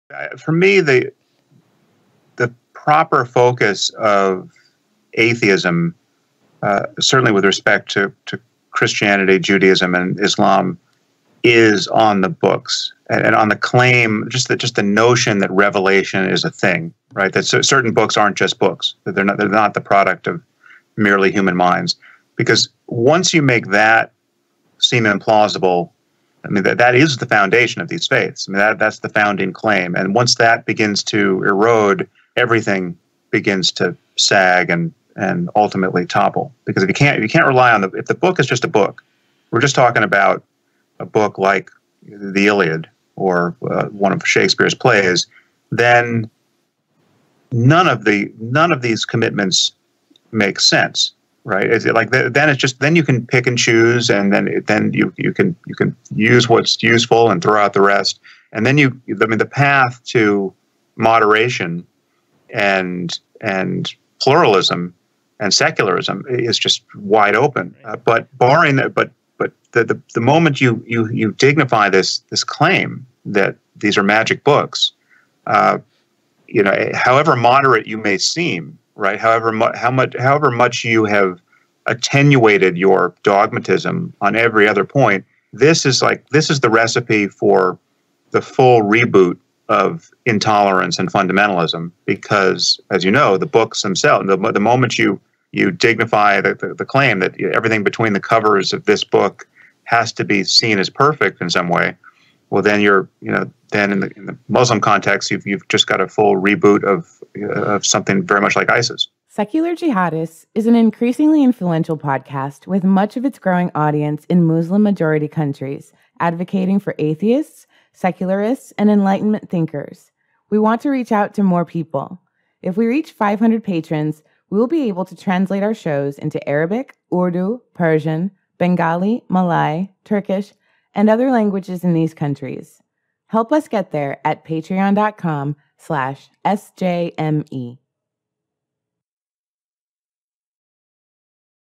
The Cognitive Foundations of Religion
An engaging conversation with Sam Harris.
In 2004, Sam Harris published his award-winning first book, "The End of Faith"—a scathing attack on religion and superstition that helped launch the so-called "New Atheist" movement along with later books by Richard Dawkins, Christopher Hitchens, and Daniel Dennett. It was the first of a string of bestsellers for Sam, which include Islam and the Future of Tolerance, his dialogue with Muslim reformer Maajid Nawaz. Today, Sam hosts Waking Up, one of the most popular podcasts in the world. He has also been a committed, unwavering supporter of freethinkers from the Muslim world through the years. We talked to him about Islam, Islamic reform, Muslims, the Regressive Left, the rise of Trump, the ex-Muslim movement, and more. Check it out.